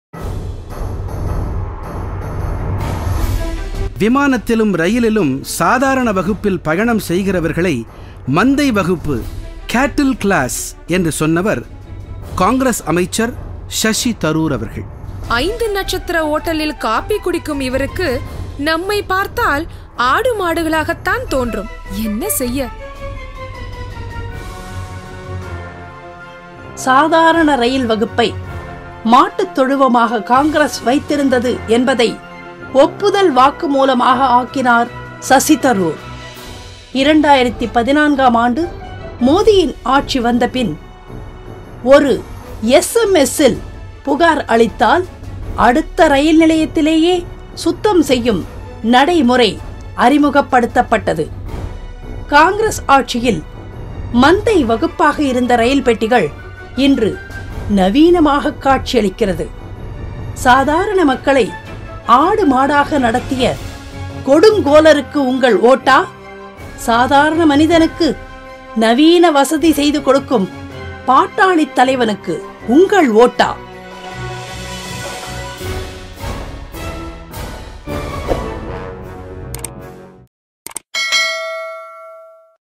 அன்றும்... இன்றும் ... - ஏழையின் மதிப்பு
சாதாரண மக்களை ஆடு மாடாக நடத்திய கொடுங்கோலருக்கு உங்கள் ஓட்டா?

சாதாரண மனிதனுக்கு நவீன வசதி செய்து கொடுக்கும் பாட்டாளித் தலைவனுக்கு உங்கள் ஓட்டா?